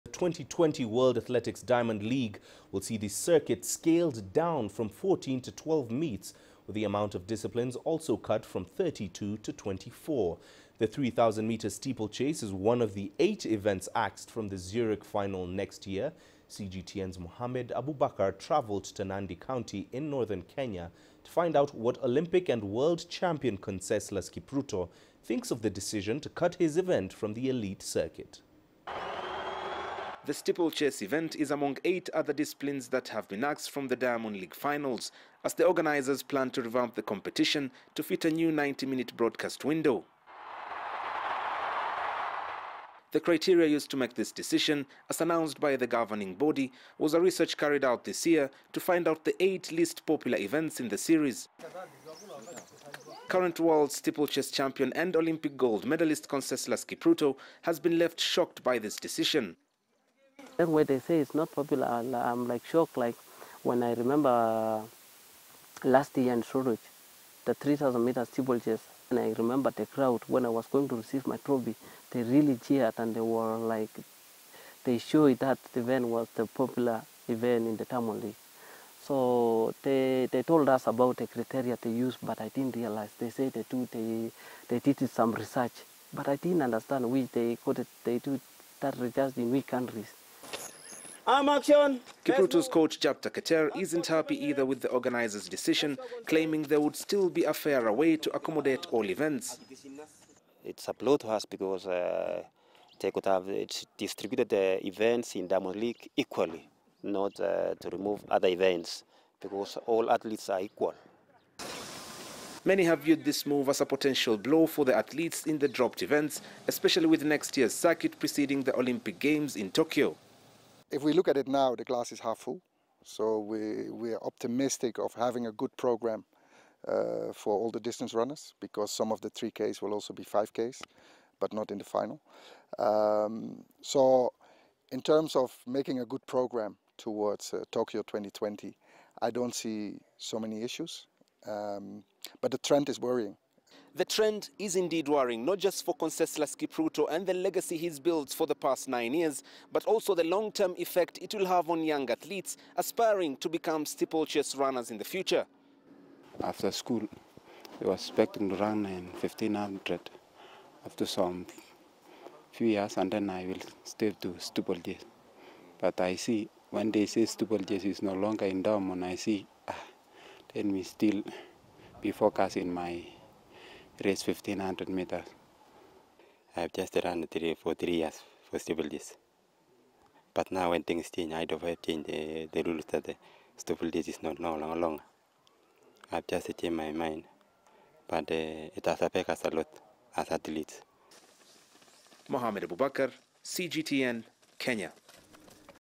S/chase champion Kipruto reacts to decision to axe his event
The 2020 World Athletics Diamond League will see the circuit scaled down from 14 to 12 meets with the amount of disciplines also cut from 32 to 24.  The 3,000 metres steeplechase is one of the eight events axed from the Zurich final next year.  CGTN's Mohamed Abubakar travelled to Nandi County in northern Kenya, to find out what Olympic and world champion Conseslus Kipruto, thinks of the decision to cut his event from the elite circuit.